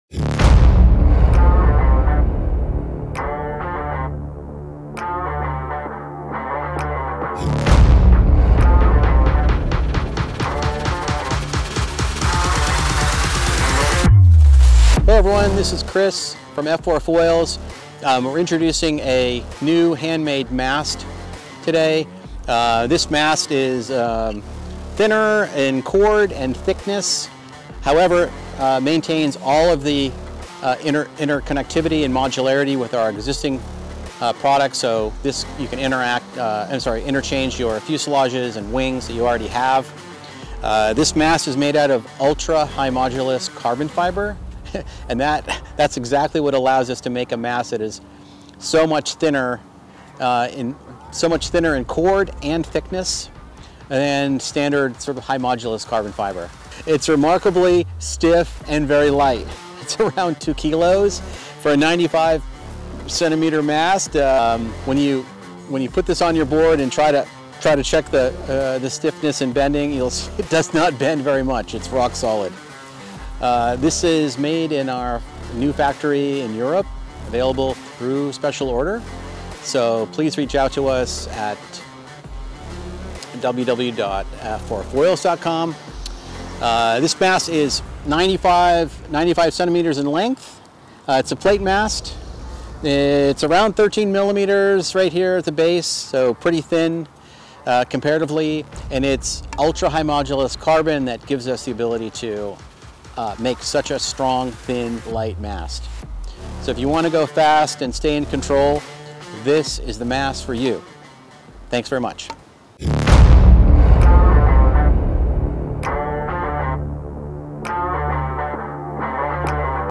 NEW GP-R HANDMADE CARBON PLATE MAST
F4 recently announced new production facilities in Europe to produce windsurfing and windwinging hydrofoils and along with that we are excited to announce the all new F4 GP-R Plate mast. The all new mast has a smaller cord, and is thinner, lighter, stiffer, and longer, resulting in much better feedback, stability, significantly reduced drag, and much higher speeds. This mast is only available as our EU hand made R version! These masts are made by hand in our EU factory using a new ultra high modulus carbon fiber with high modulus resins and will be available through special order.

Our EU handmade GP-R Carbon Plate Mast weighs around 2.2 kg, which is extremely light for a solid carbon mast. It is 95 cm long and optimized for torsional bending stiffness, excellent for chasing new levels of maneuverability, stability, and breaking speed records.

We have maintained all the modular features of our current carbon fiber and aluminum tuttle and plate production masts so you can use it with any fuselage and wings across the windsurfing and windwinging F4 foil range.

Features
– All new thinner and stiffer construction using ultra high modulus carbon fiber and resins
– Optimized profile sections for ventilation resistance, stiffness, and performance
– Shorter cord for reduced wetted surface
– Modular design that fits F4 fuselages for windsurf and wingboard foils

With the GP-R mast you feel an immediate difference, it's fast and slippery and thanks to its superior stiffness it gives the rider amazing control. No speed wobbles and no torsional bending during turning delivers better control for all aspects of foiling – wave riding, gliding, jumping, freestyle and race.

The GP-R mast is available immediately for special order through our certified resellers worldwide and through our website.

See you on the water,
F4 Team 🤙